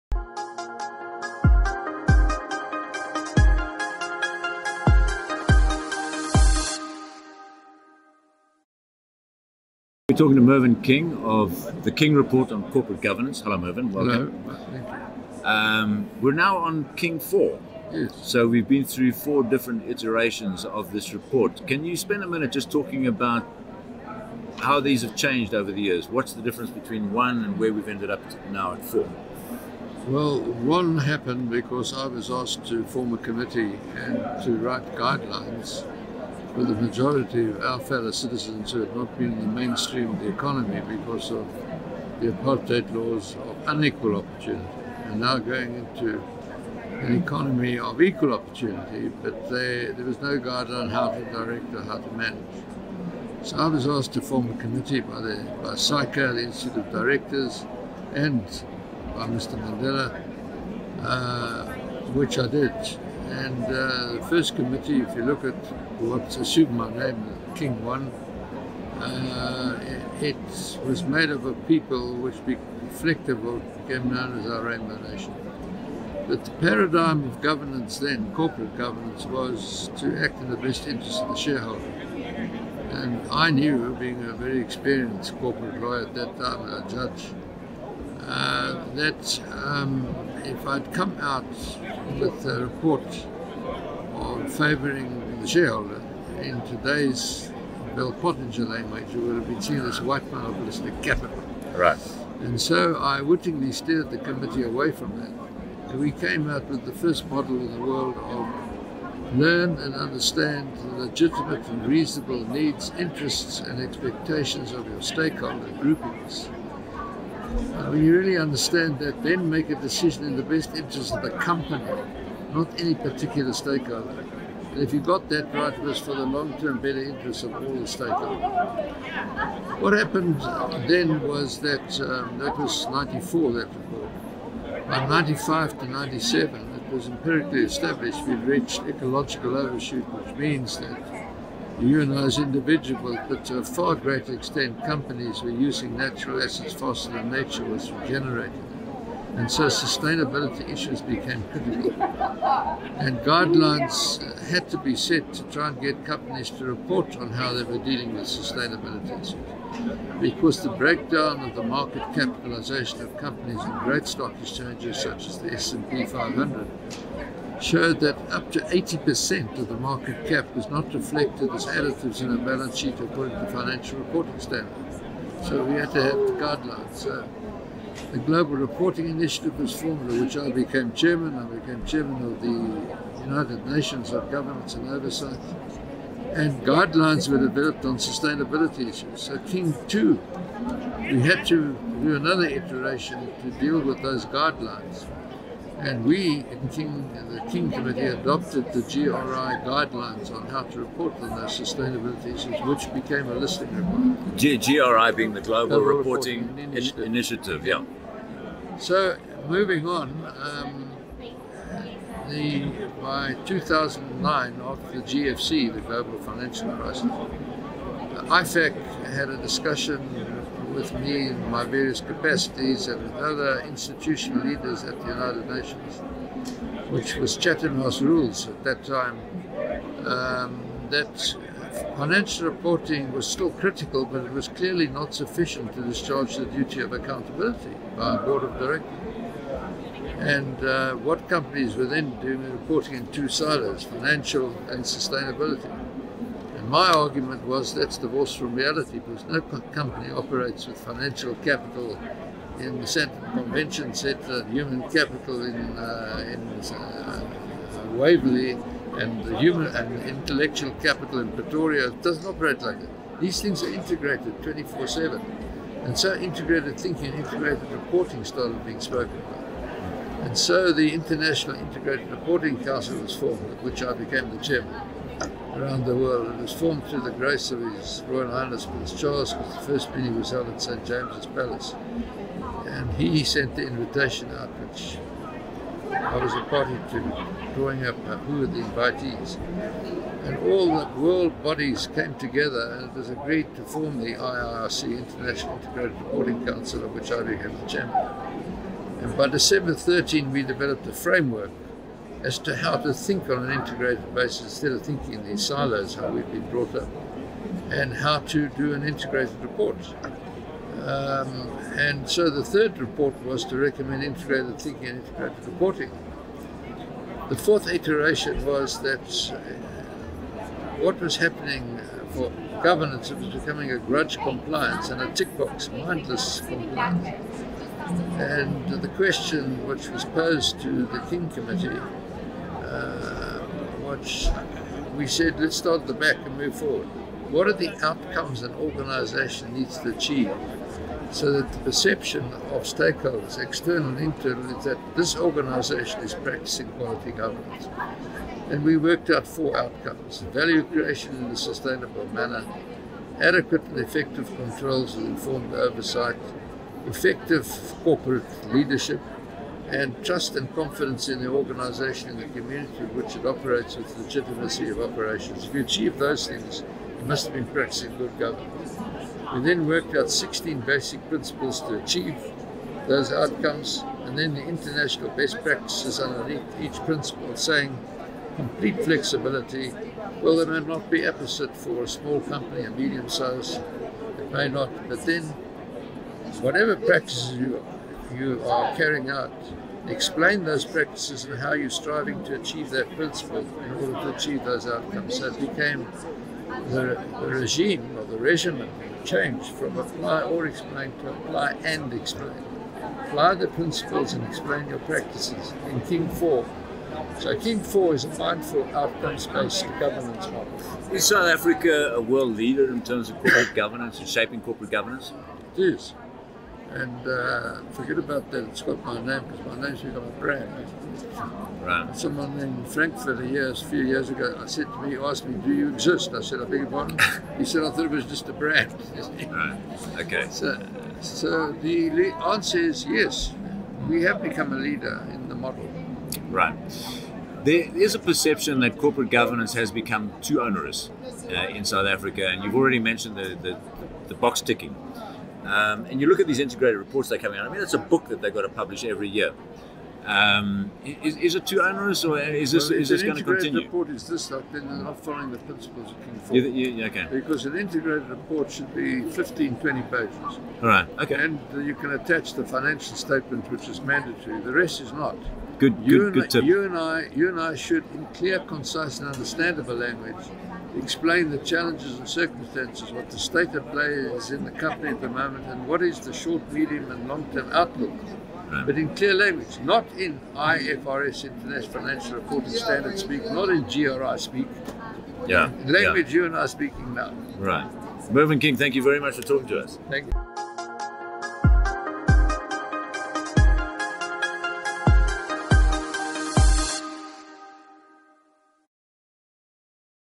Mervyn King of the King Corporate Governance Reports
CFO Talks interviews Mervyn King, former judge, author at Leaderex 2019